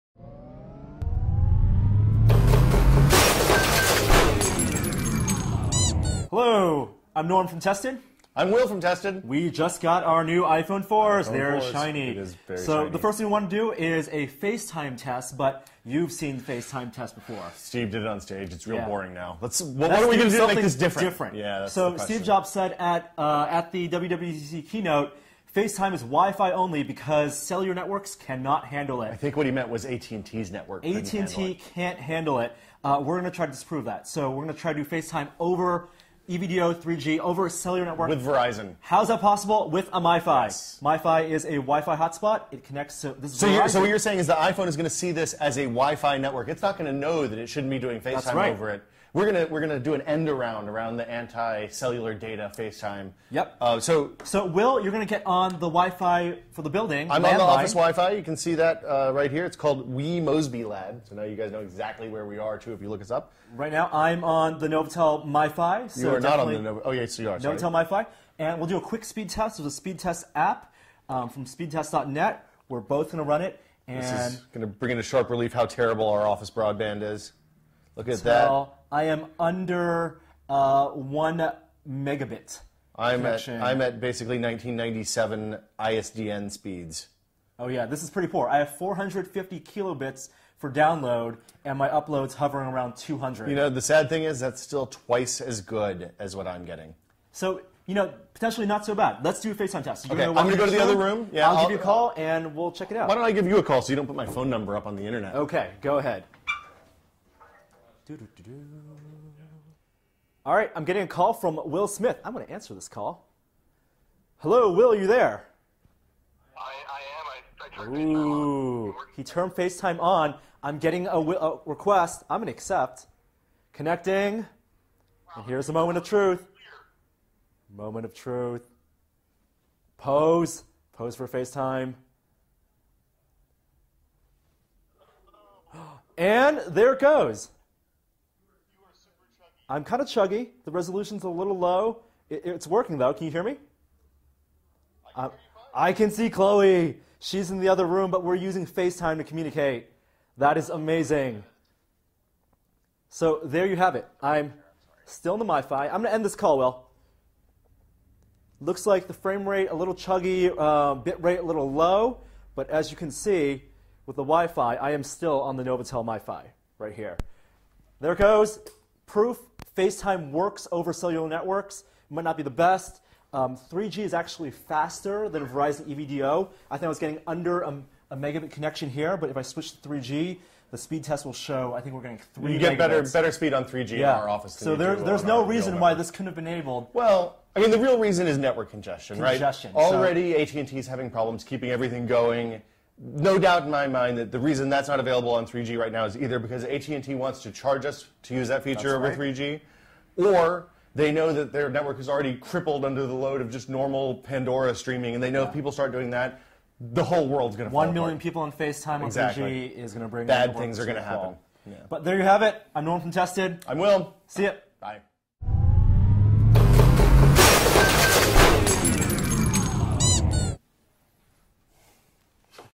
Here is iPhone 4 FaceTime Working on a Cellular Network
Steve Jobs said cellular networks weren't ready for FaceTime. Turns out, it works just fine.